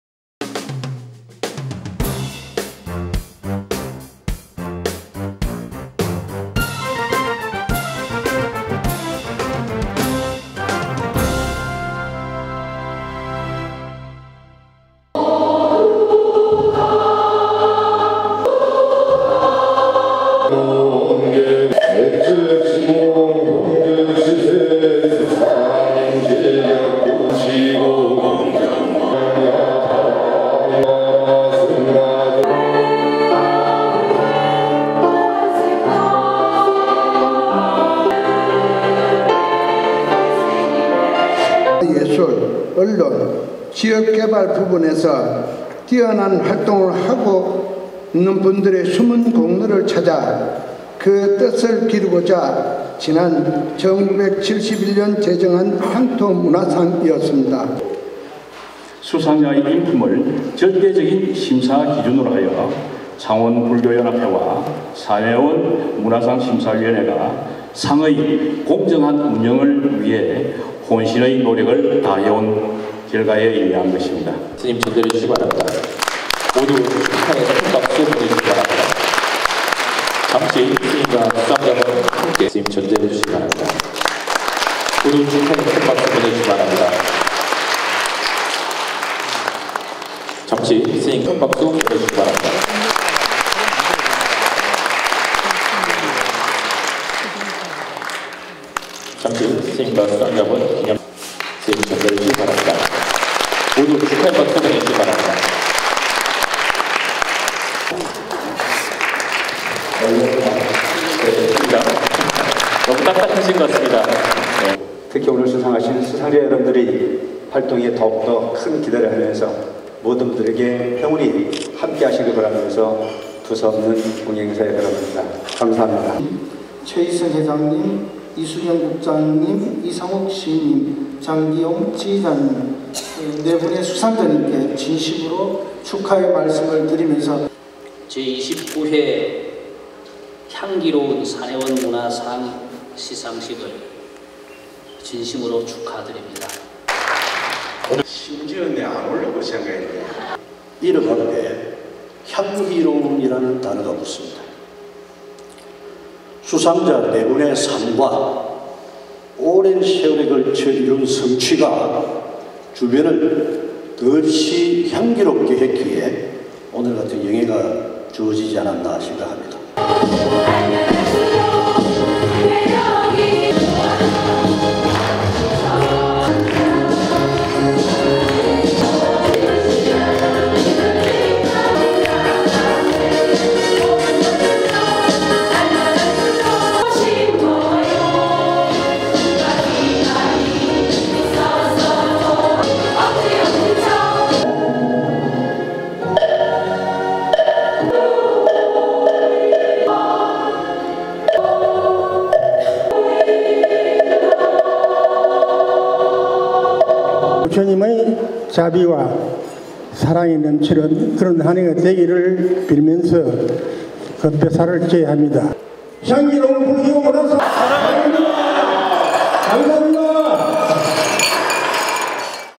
제29회 향기로운 산해원문화상시상식
창원시불교연합회(회장 월도 스님)는 14일 제29회 (향기로운)산해원문화상 수상자 4명을 선정해 2019년 12월 9일 그랜드 머규어 앰버서더 창원 에서 시상식을 가졌다

올해는 특별상(지역개발)에 최위승 무학그룹 명예회장, 문학상에 이상옥 한국디카시연구소 대표, 예술상에 장기홍 합창단 지휘자, 언론상에 이수경 경남도민일보 자치행정부 국장이 각각 수상자로 선정됐다.